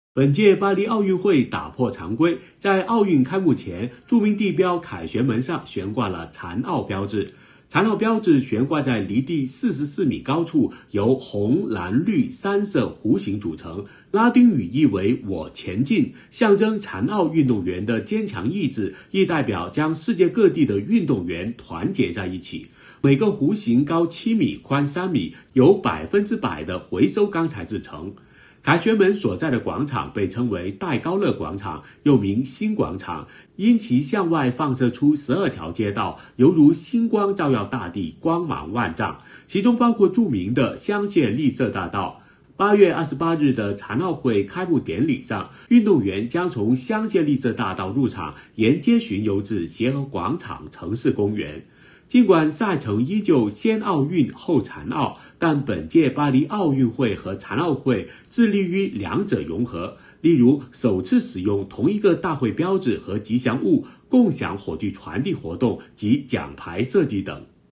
巴黎奥运会打破常规！残奥标志开幕前在凯旋门惊艳亮相
**Les Jeux Olympiques de Paris brisent les conventions ! Le symbole des Jeux Paralympiques fait une apparition spectaculaire sur l'Arc de Triomphe avant la cérémonie d'ouverture.**

**Paris Olympics Break the Mold! Paralympic Symbol Makes a Stunning Appearance on the Arc de Triomphe Before the Opening Ceremony.**

1.
2.
3.
4.
5.
6.
7.
8.
9.

1.
2.
3.
4.
5.
6.
7.
8.
9.

1.
2.
3.
4.
5.
6.
7.
8.
9.